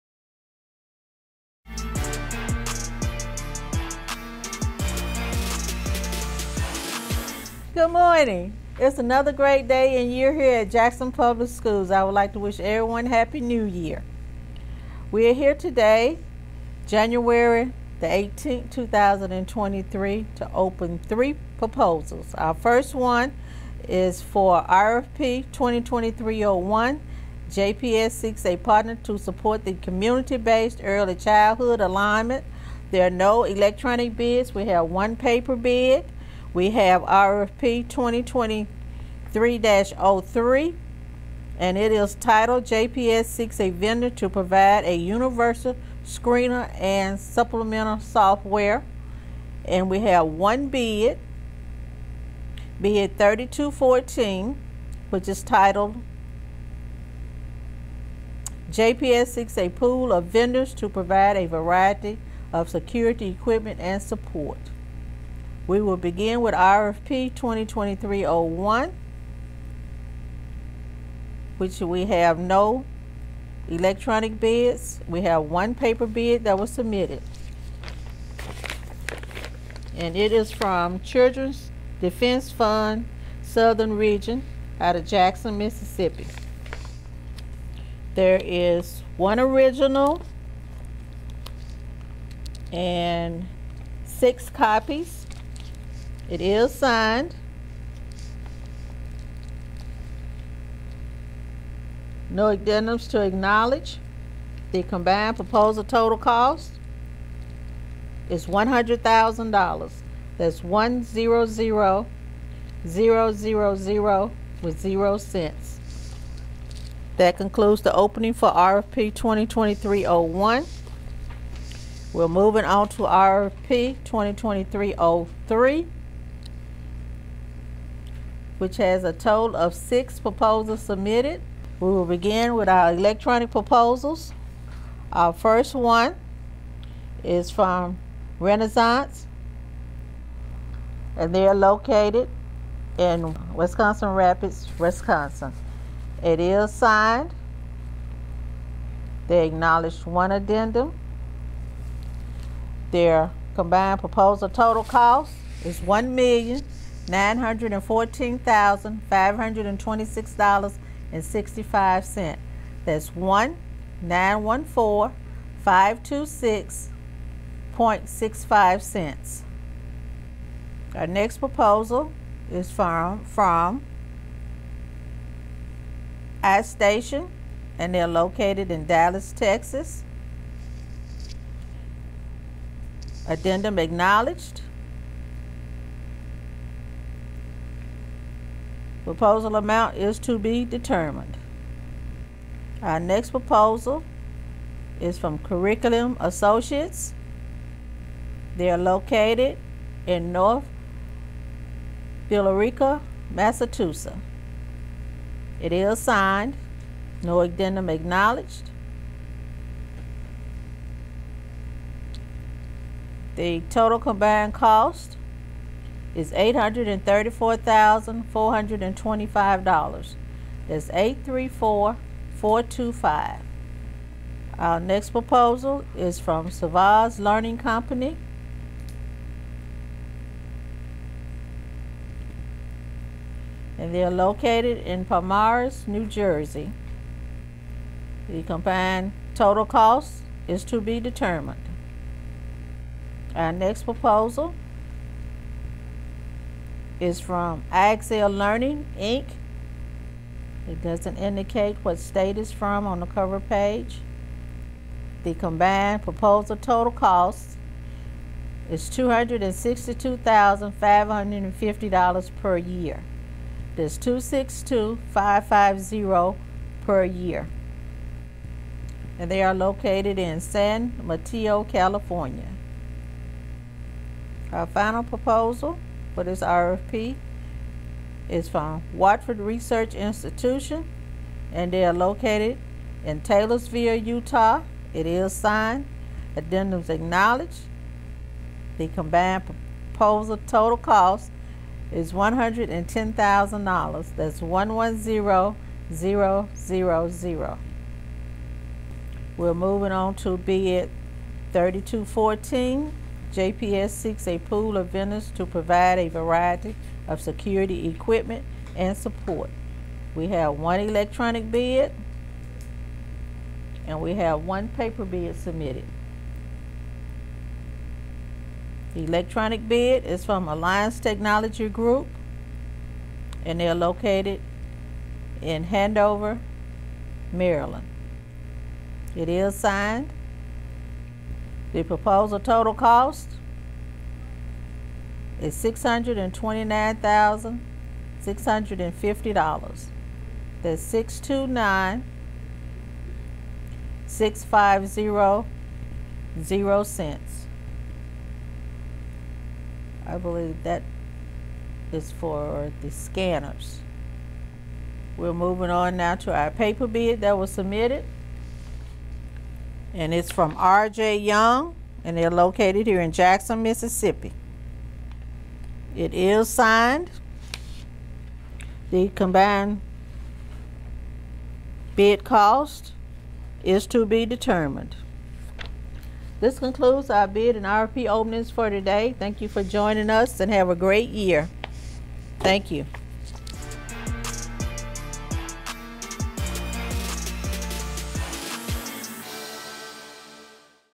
Bids/RFP Openings x 3 for 1/18/23.
Jackson Public School District, Jackson, MS.Bids/RFP Openings x 3 for 1/18/23.
RFP 2023-01
Bid 3214
RFP 2023-03